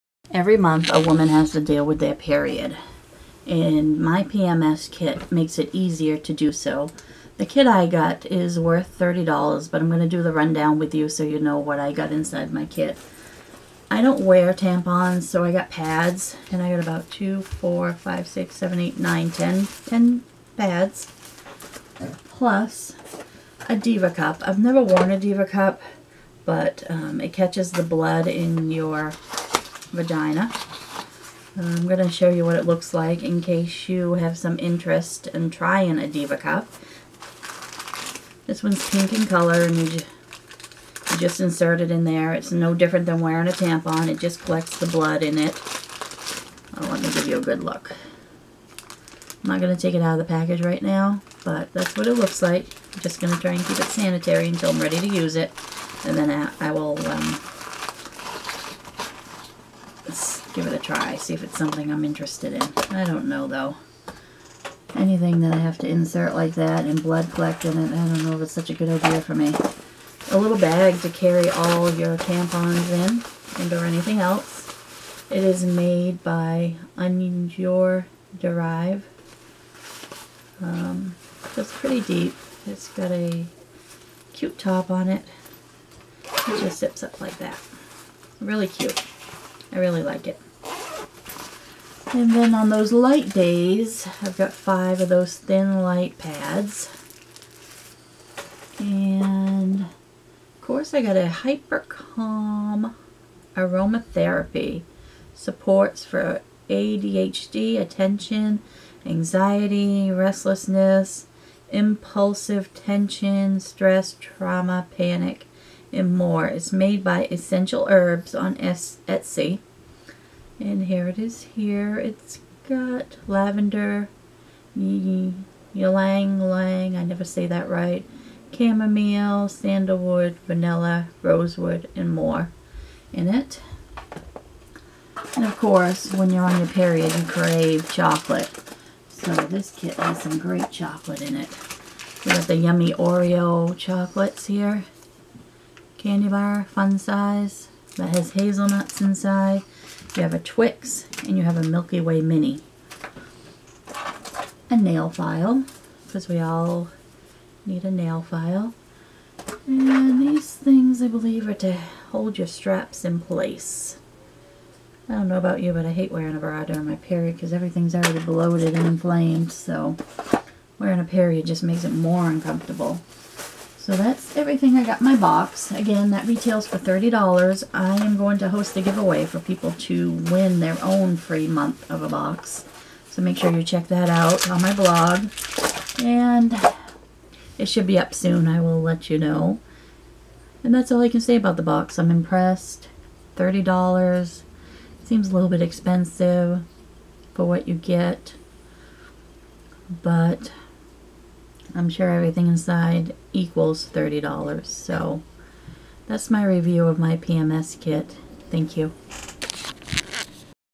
My PMS Kit
My Review for the monthly PMS Kit. I am hosting a giveaway as well on my blog which ends 7/31which is